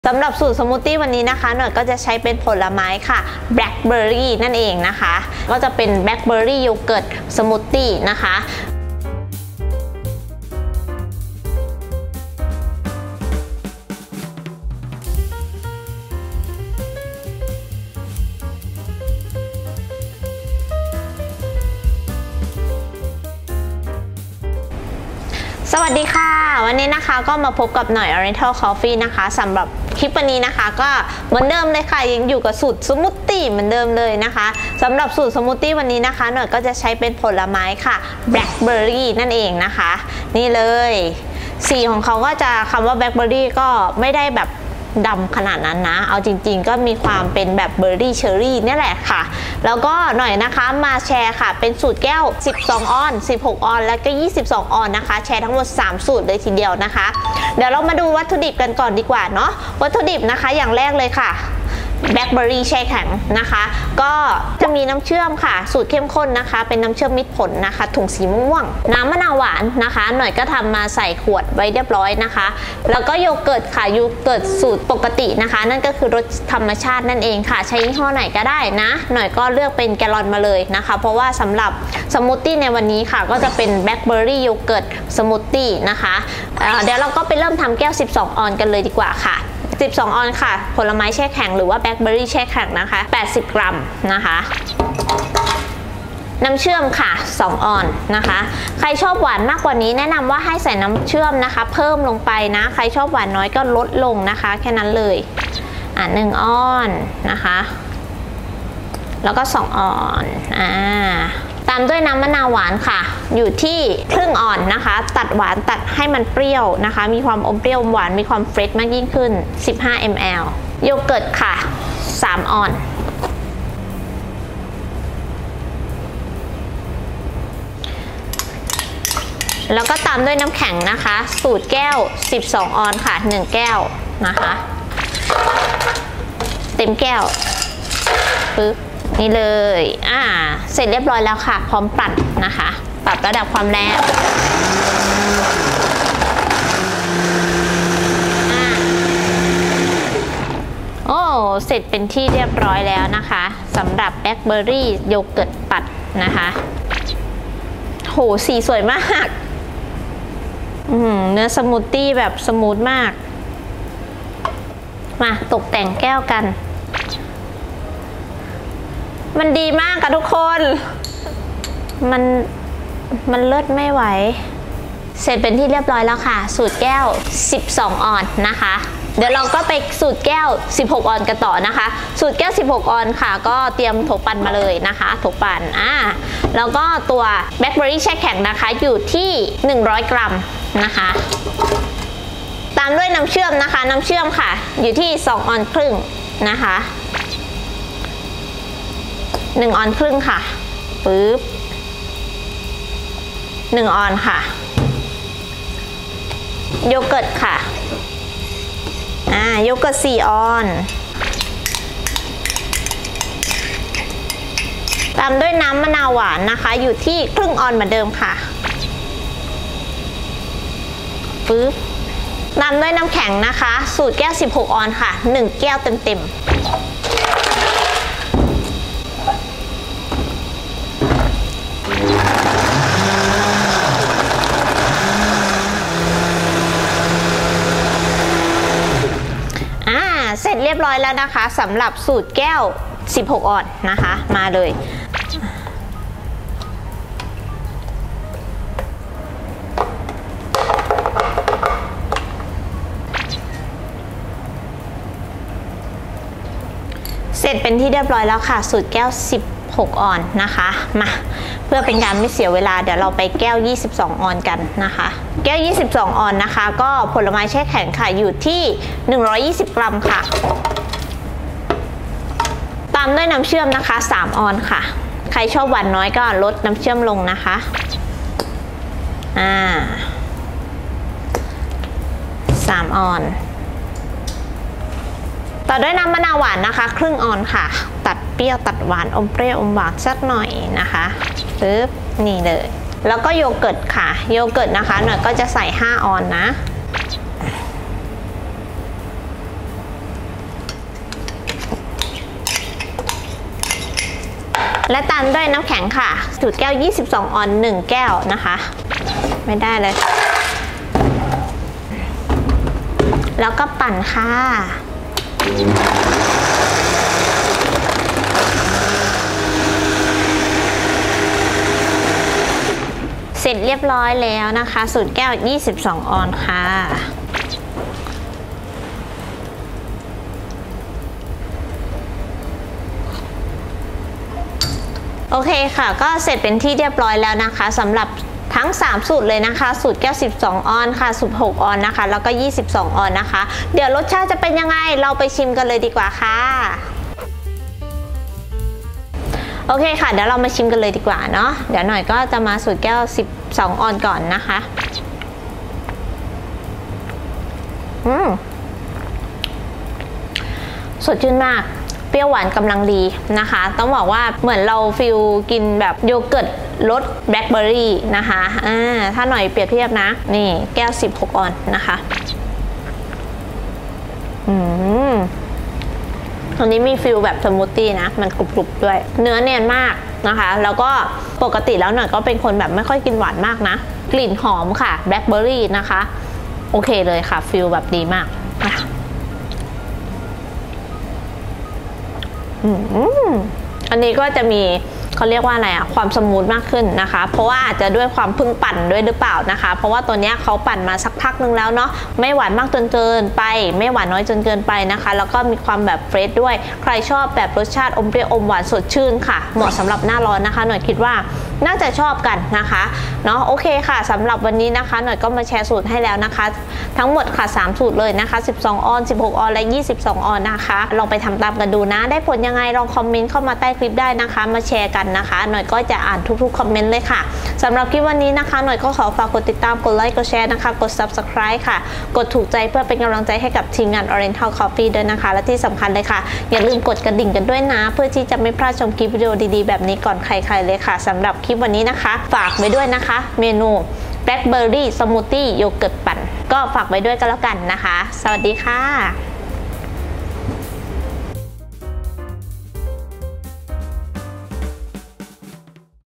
แชร์สูตร แบล็กเบอร์รีสมูทตี้แก้ว 12 oz. / 16 oz. / 22 oz. สูตรนี้ทำตามได้ง่ายๆ (Blackberry Smoothie)
ส่วนผสมแก้ว 12 ออนซ์
🫐 แบล็กเบอร์รี 80 กรัม
🫐 โยเกิร์ตรสธรรมชาติ 90 มล.
🫐 น้ำมะนาวหวาน 15 มล.
🫐 น้ำเชื่อม 60 มล.
🫐 น้ำแข็ง 1 แก้ว 12 ออนซ์

ส่วนผสมแก้ว 16 ออนซ์
🫐 แบล็กเบอร์รี 100 กรัม
🫐 โยเกิร์ตรสธรรมชาติ 120 มล.
🫐 น้ำเชื่อม 75 มล.
🫐 น้ำแข็ง 1 แก้ว 16 ออนซ์

ส่วนผสมแก้ว 22 ออนซ์
🫐 แบล็กเบอร์รี 120 กรัม
🫐 โยเกิร์ตรสธรรมชาติ 150 มล.
🫐 น้ำเชื่อม 90 มล.
🫐 น้ำแข็ง 1 แก้ว 22 ออนซ์

✨สามารถปรับสูตรความหวานตามชอบได้เลยนะคะ
ติดตามข่าวสาร โปรโมชั่นต่างๆ ได้ที่
📞โทร.093-5569962
📞 โทร.064-3626549